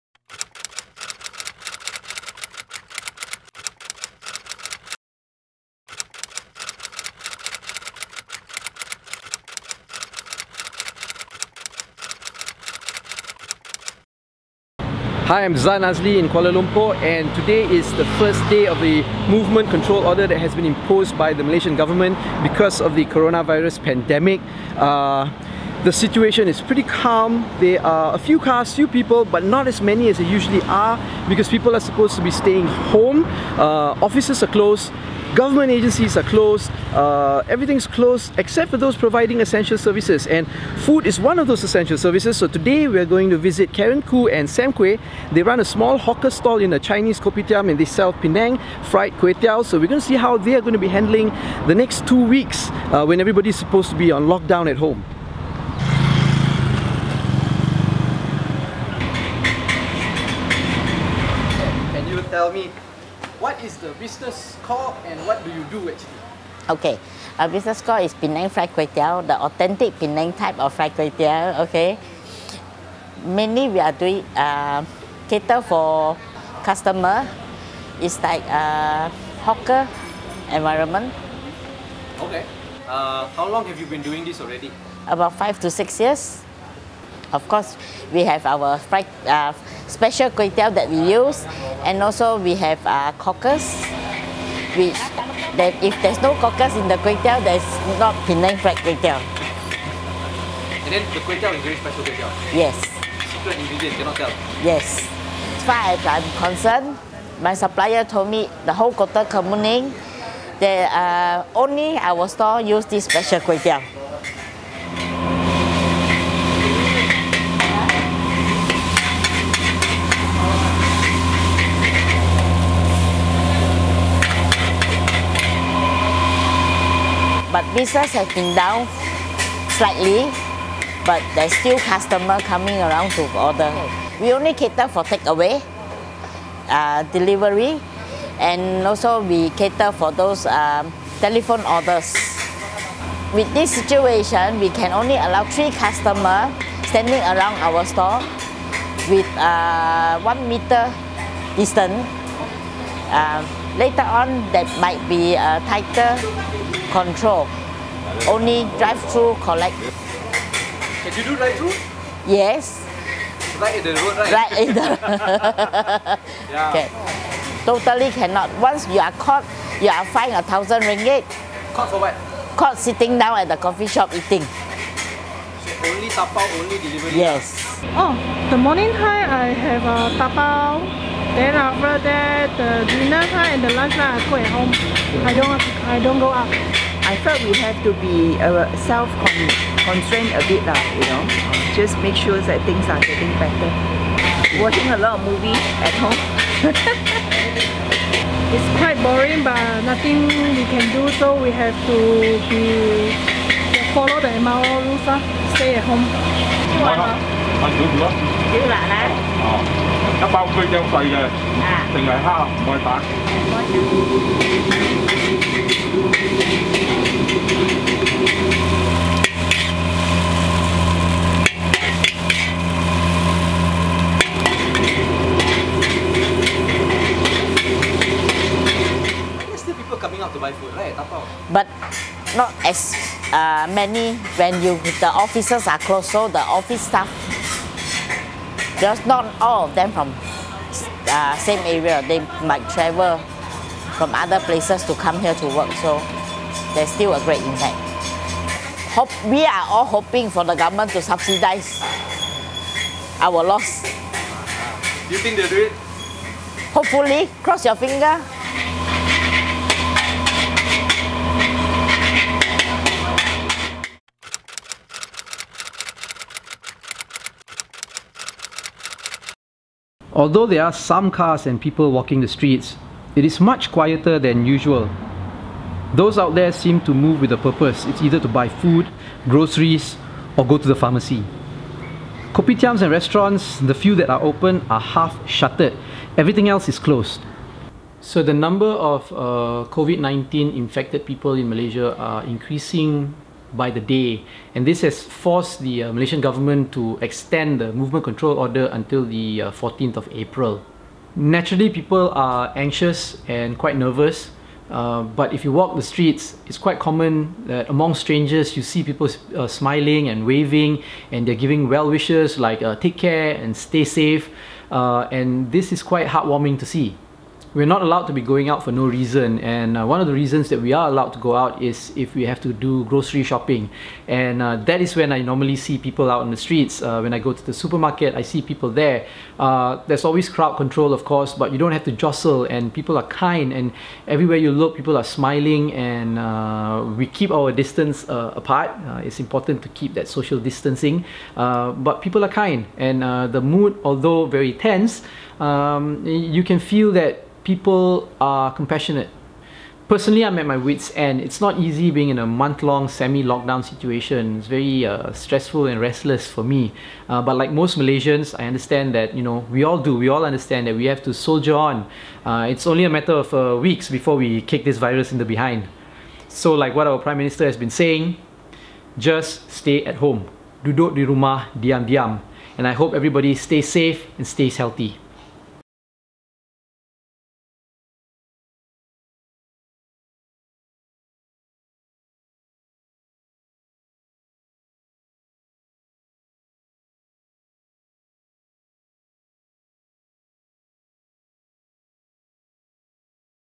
F&B Outlets Struggle With Malaysia's Lockdown | Facing Covid-19
Malaysia is currently in the midst of a month long semi-lockdown due to the global Covid-19 pandemic.

Offices, schools and shops are closed except for those providing essential services like food due to the movement control order. We visit a small fried kuih teow hawker stall to see how Malaysia hawkers are adapting to the lockdown.

This video is part of our Neighbours series. We walk past them every day, not realising that even the most ordinary of people have extraordinary stories to tell. Hear them speak.